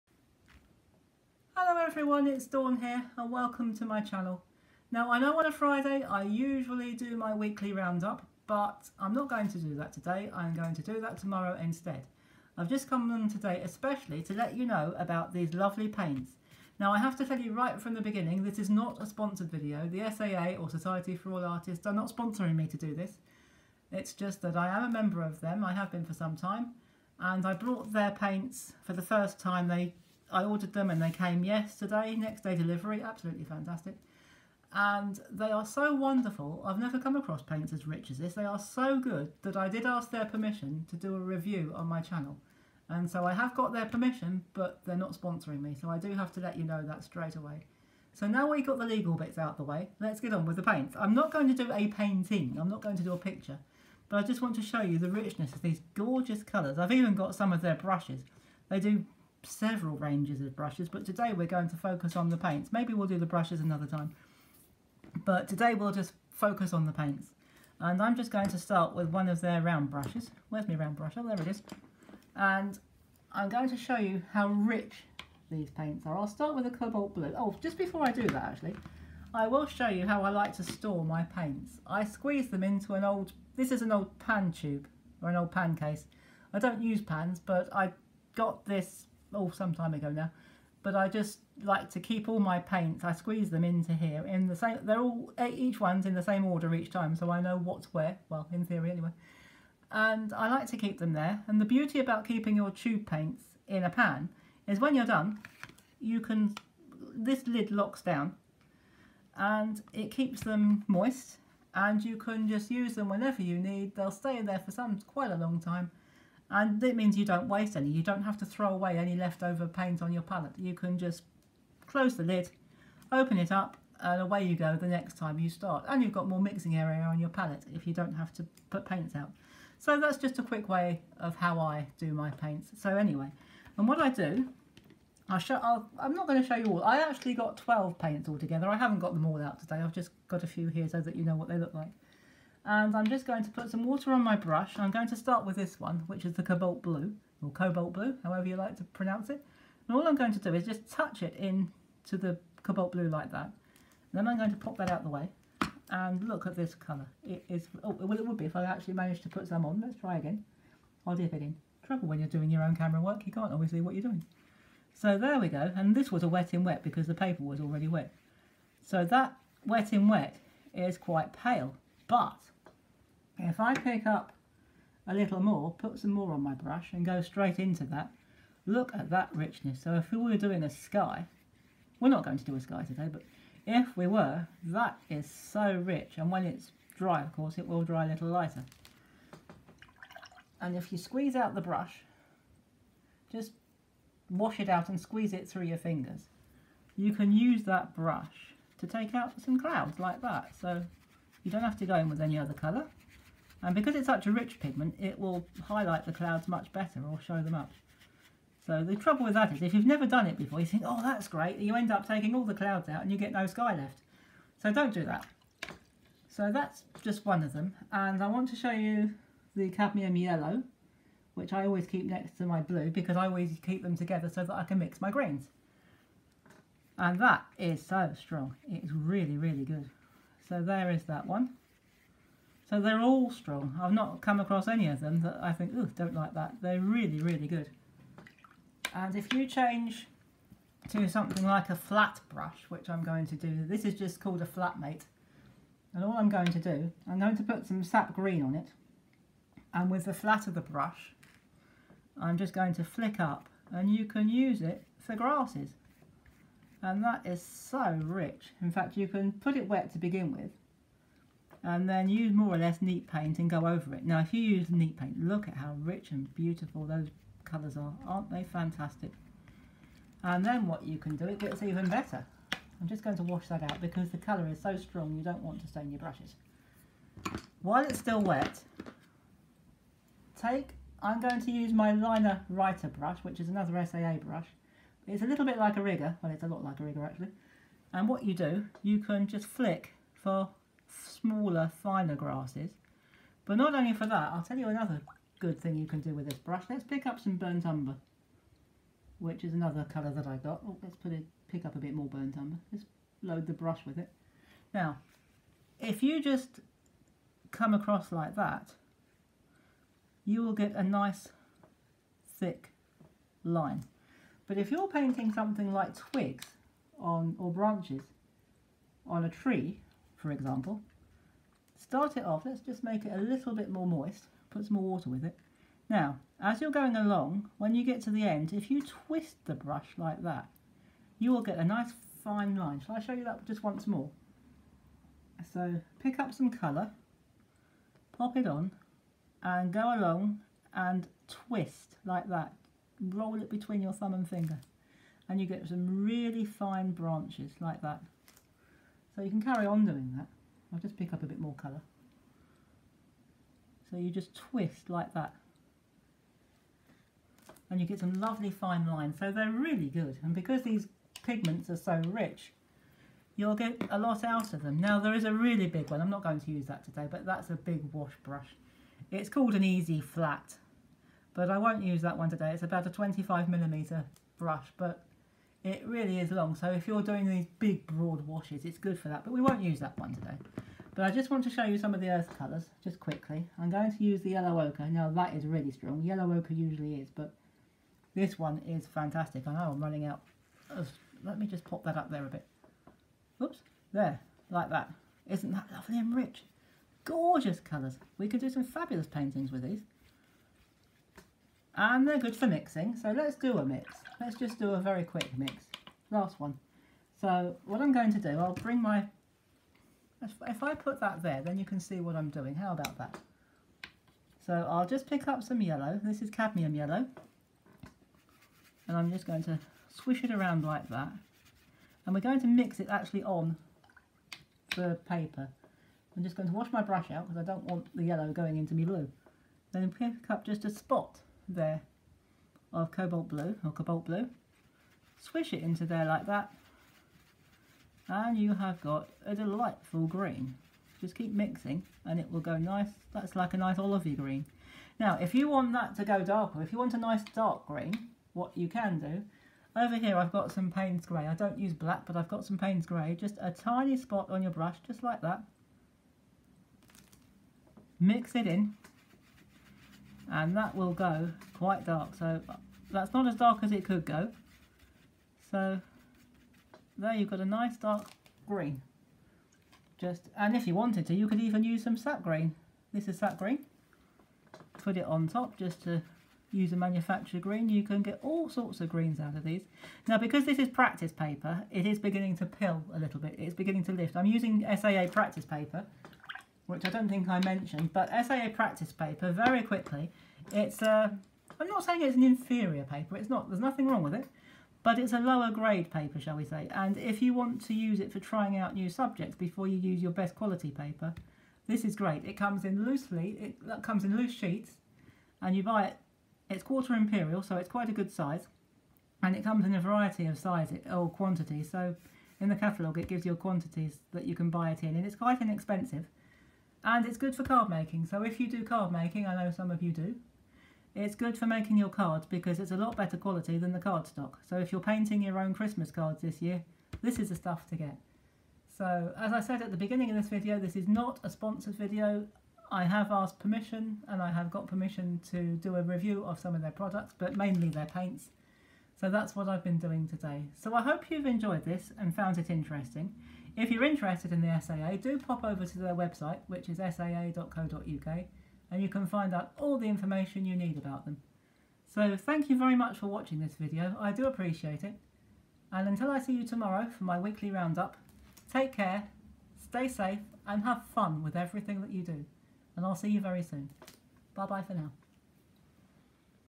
Paint Review
The paints that I used in this review are the own brand artist watercolours from the Society for All Artists. They are supplied in a fifteen millimetre tube and although they are artist quality paints, they are all the same price. The colours are luscious and strong and can produce some lovely paintings and effects. So far I’ve only tried these paints out for this review, but I’m really looking forward to doing a proper painting with them. In fact what I might do is to do a painting with these paints and the same painting with my precious paints and observe the differences. I’ll share any discoveries that I find with you.
I hope you like the review and find it helpful.
If you are interested in buying these paints, you need to be a member of the SAA and you’ll find all of the information that you need, including the membership levels on their website at
saa.co.uk